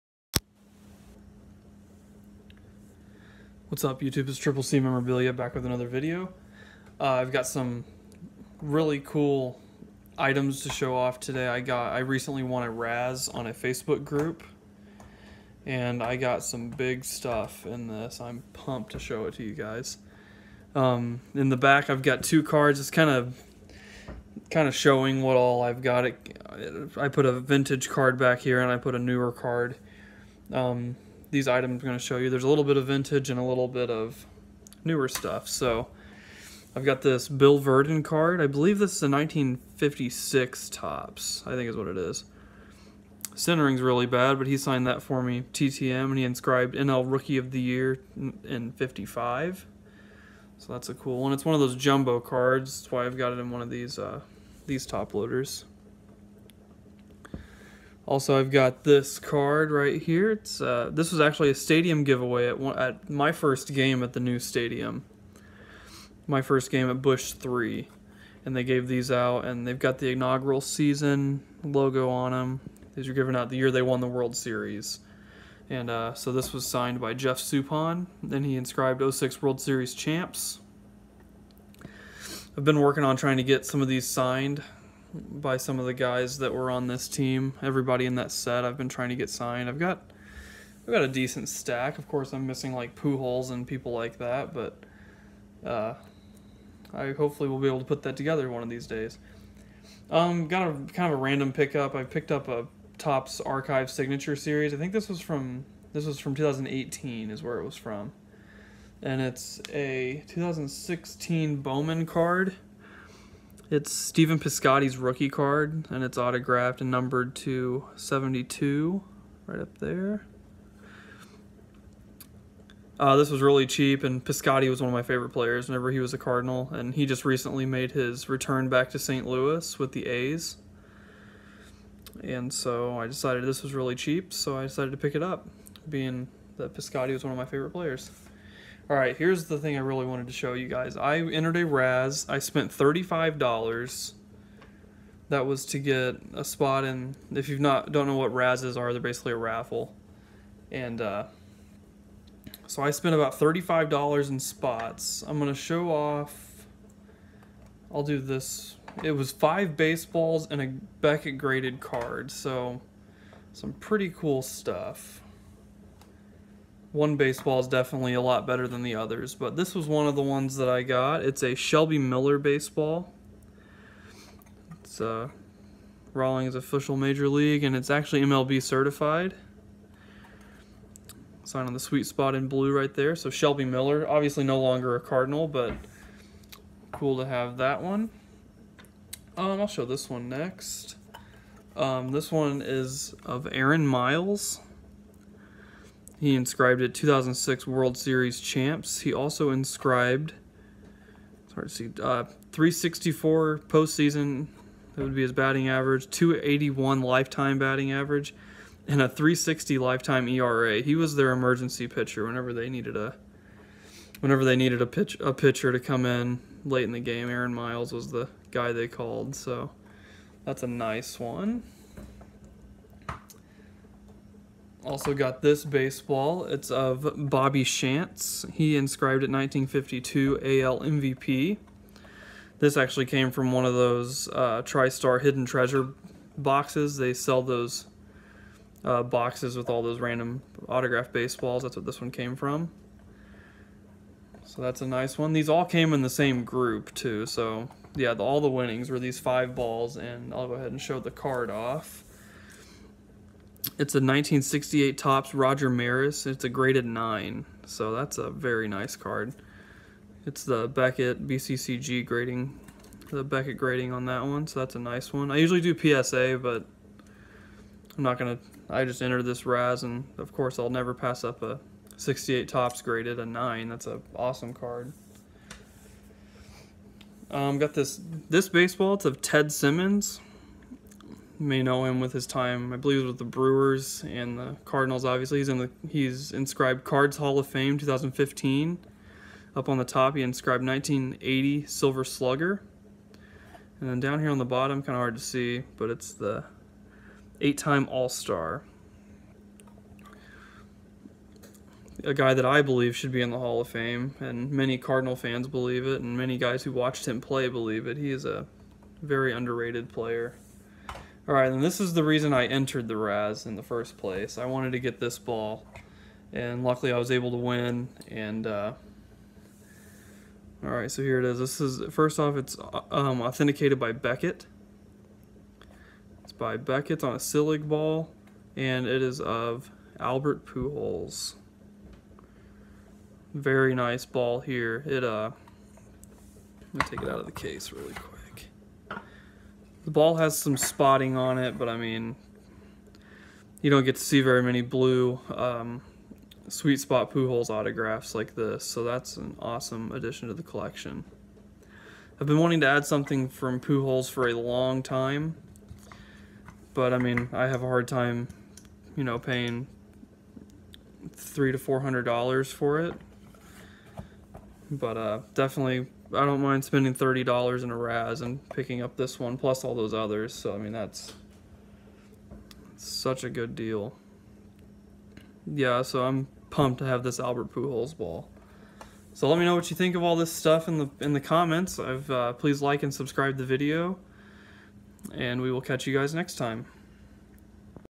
Huge St Louis Cardinals Razz Win!
Won a few signed baseballs in a razz! Great stuff!